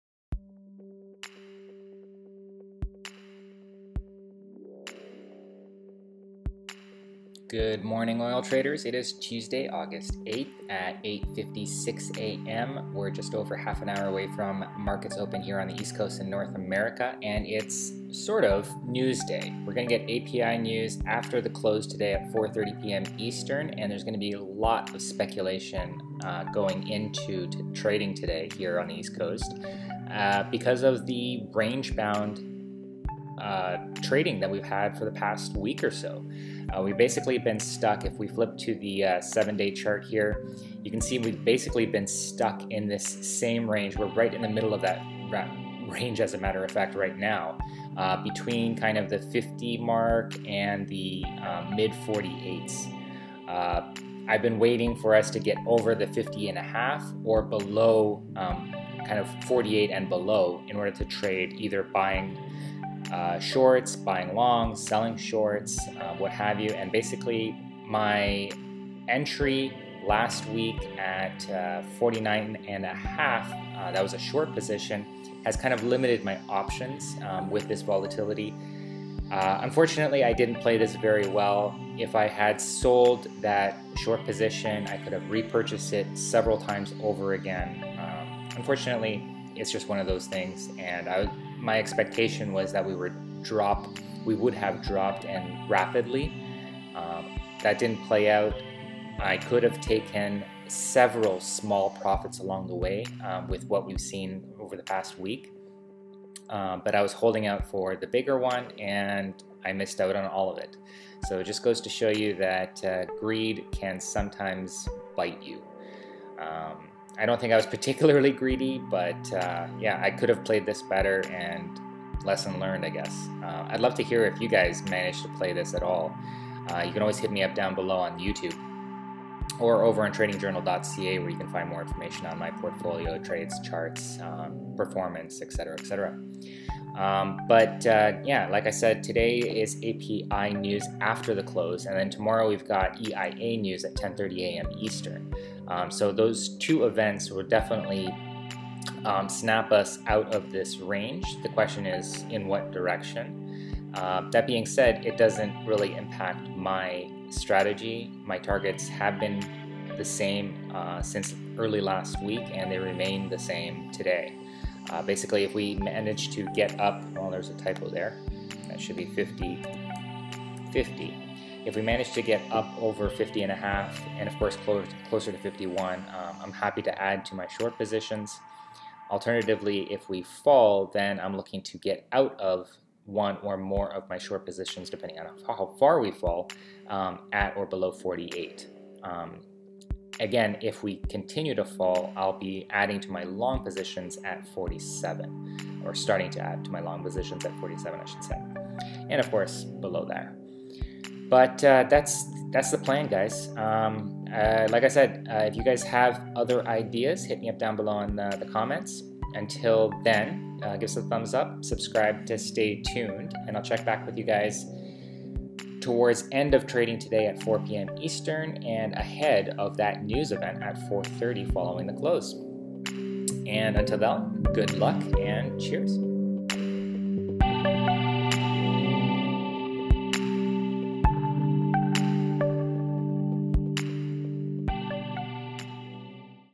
78 - Game Plan for Aug 8, 2017 - API News day and Speculation Could Break Crude Out of Range
* We’ve waited [im]patiently, but it’s news day, sort of…
    * Time to break out of our range rut
* API news today at 4:30p EST, after close, but there will be a lot of speculation about what that news will be; we should snap out in one direction or another
* Targets are unchanged
    * Add to shorts above $50.50 (nearer to $51)
    * Sell shorts at $48 and below
    * Add to longs at $47 …and below :)